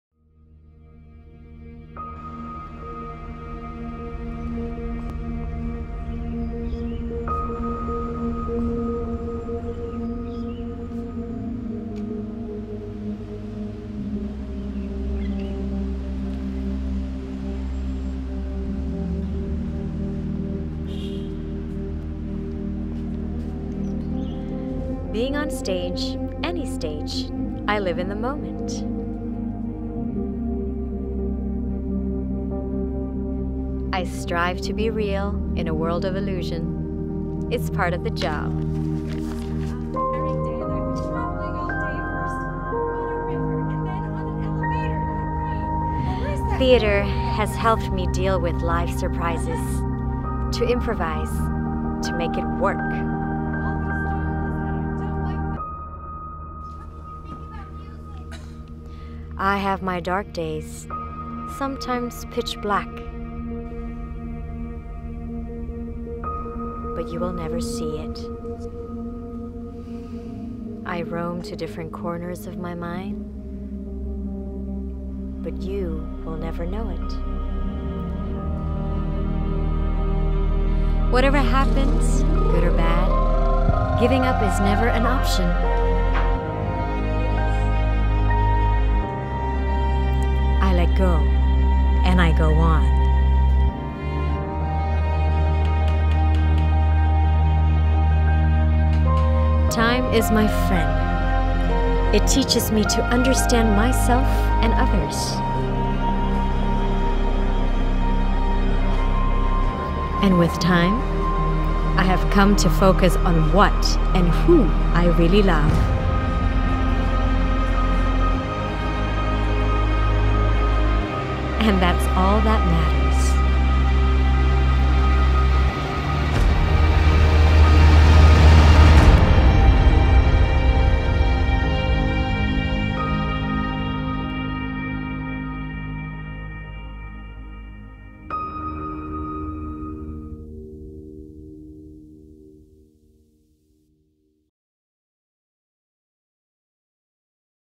Life VS Screen Marissa Anita
Publik sudah mengenal wajah Marissa Anita sebagai reporter, news anchor, presenter TV dan juga aktris film (Selamat Pagi Malam). Namun belum banyak yang tahu kiprahnya selama lebih dari satu dekade di teater. Slate mengintip Marissa dalam persiapan pementasan teater Eurydice sebagai pemeran utama.